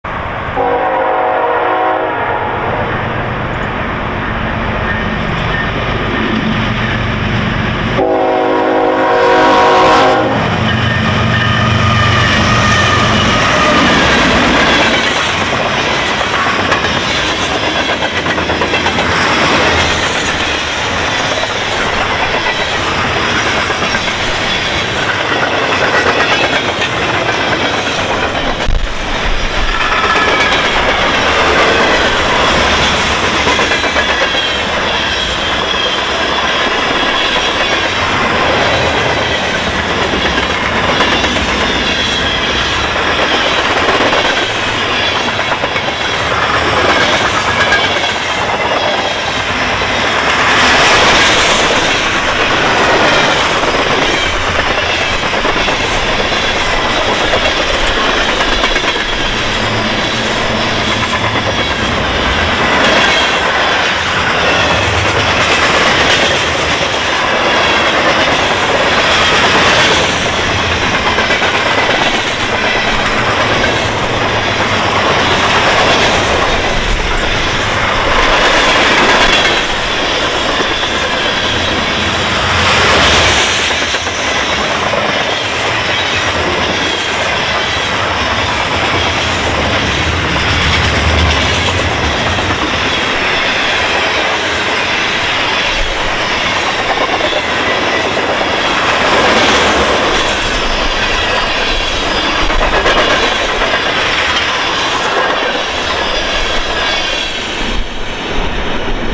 CN Train 321 Westbound January 31, 2022
Engines: CN 3912, CN 3906
Cars: 120
Location: Brockville VIA station, ON, Canada
Time: 08:23 EST
Temperature: -16C
Origin: Rivieres des Prairies QC
Destination: Toronto MacMillan ON